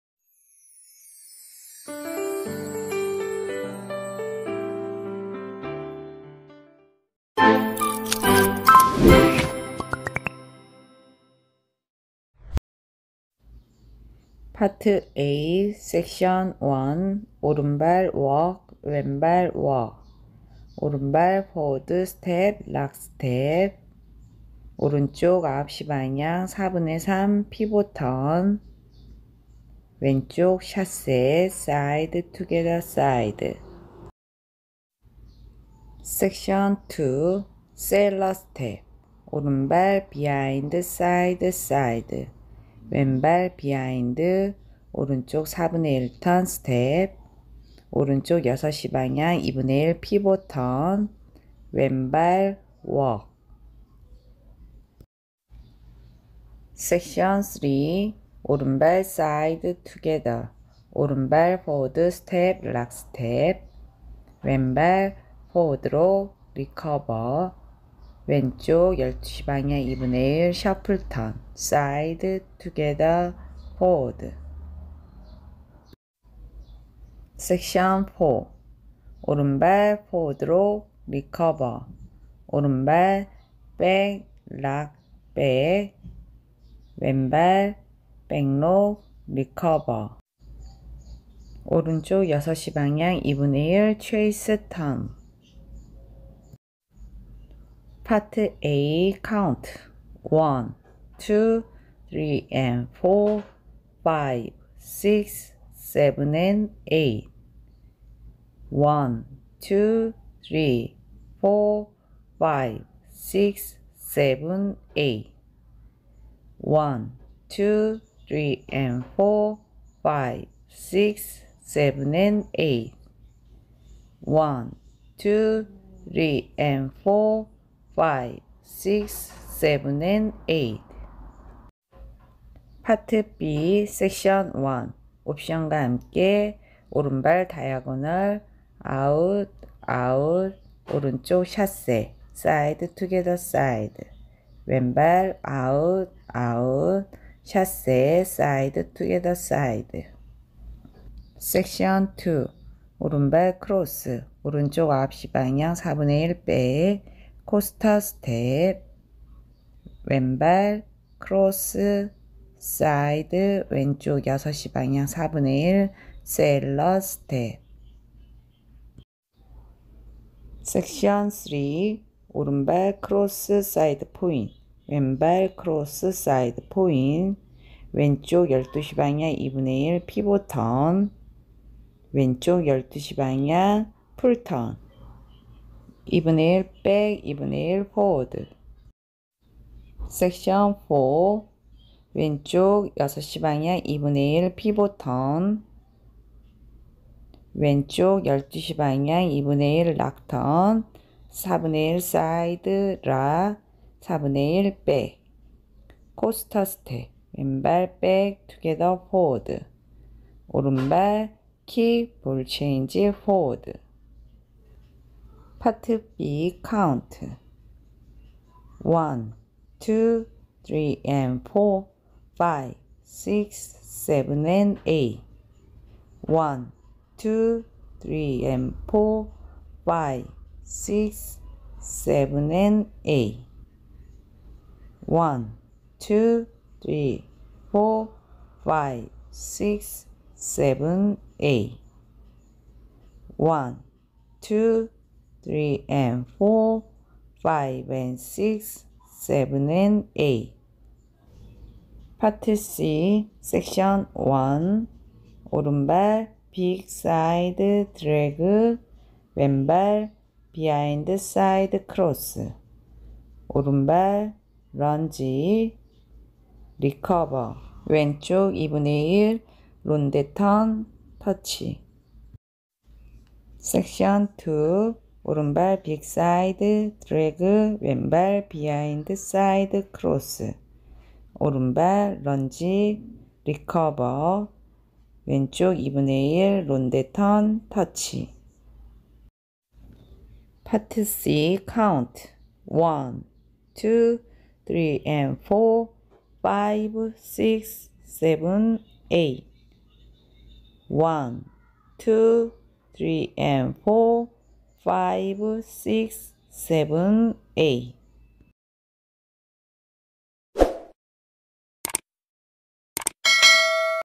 ⭐TUTORIAL/스텝설명 ⭐Thrill Me Cha Cha|80C 2W Phrased Intermediate
Count:80  Wall:2
Level:Phrased Intermediate
Choreo:Tim Johnson (UK) & Hayley Wheatley (UK) - June 2021